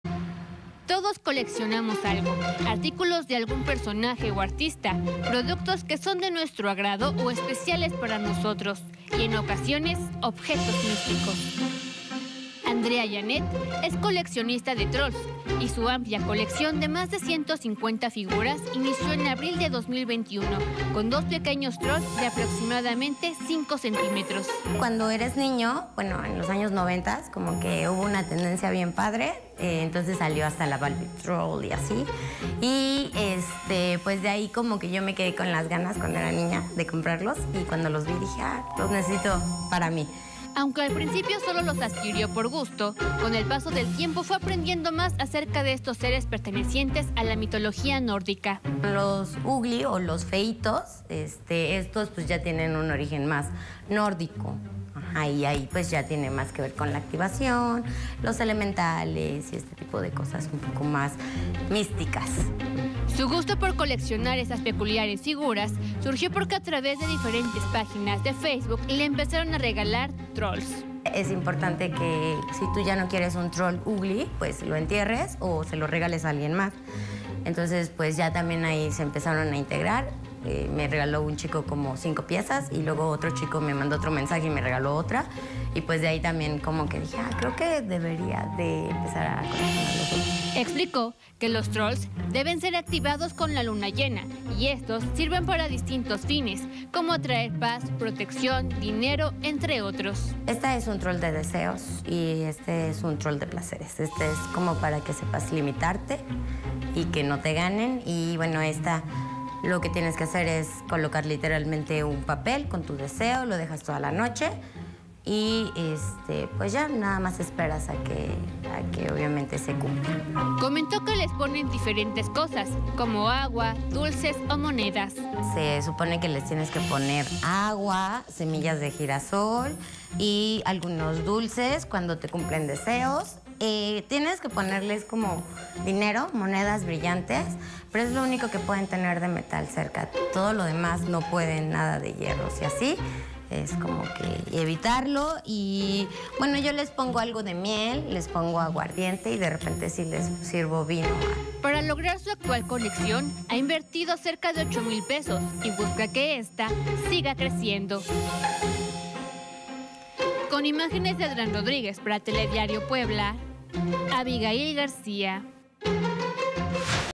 Andrea, la poblana que colecciona "trolls" y se ha vuelto experta en ellos
Andrea Janeth León es una mujer poblana que desde niña adoró los 'trolls', los famosos seres fantásticos de la mitología y folclore escandinavo. Con el tiempo se ha hecho experta en ellos, incluso aconseja que si tú tienes alguno y ya no lo quieres, debes regalarlo o enterrarlo.